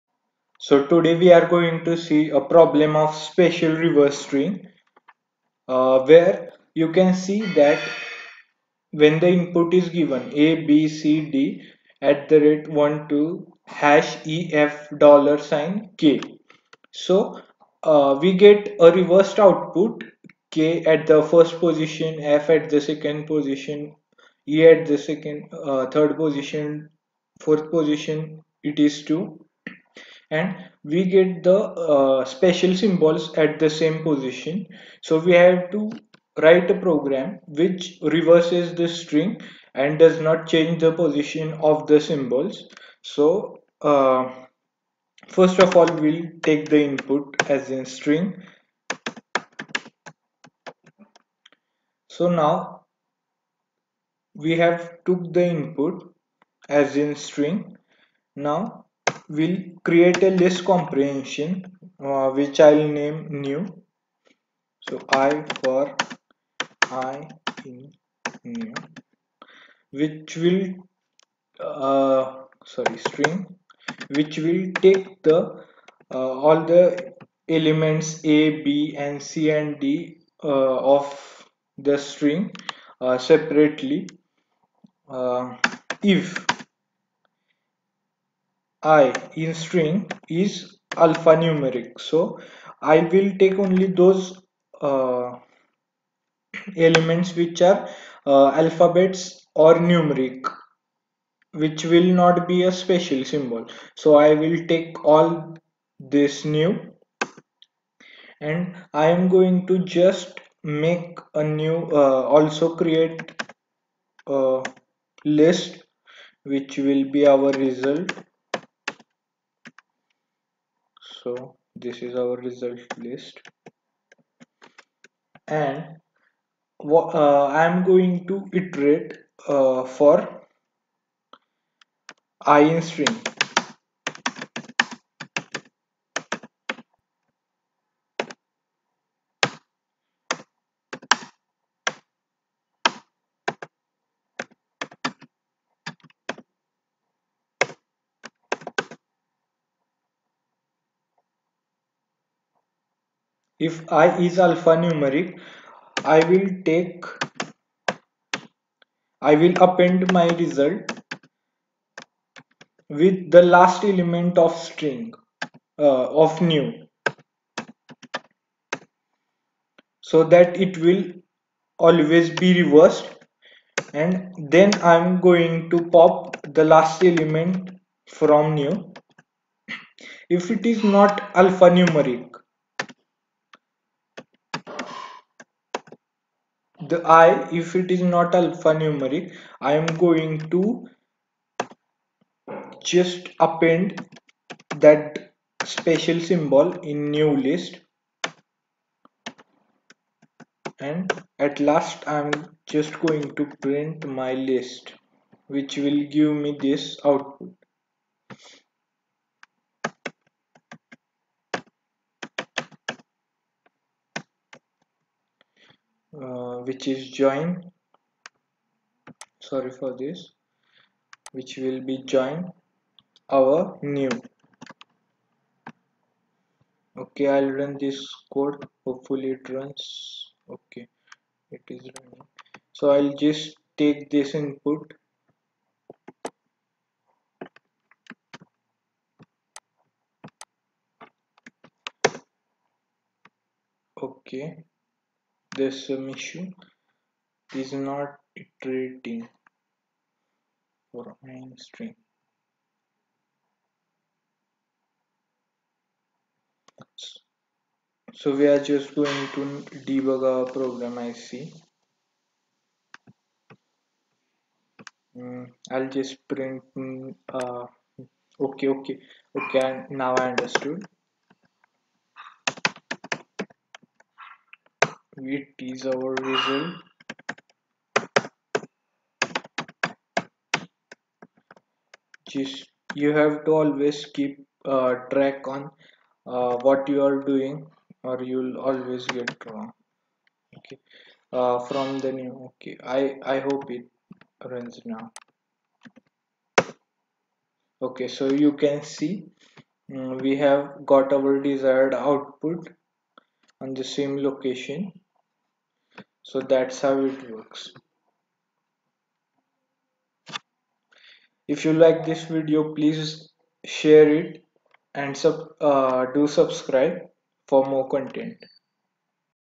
Infytq Problem | Special Reverse String | Python Solution
Infytq Examination previously asked Question series to help you Crack Infytq Coding Questions.

Sorry For this RAW VIDEO!!

Suggestions to Improve are appreciated.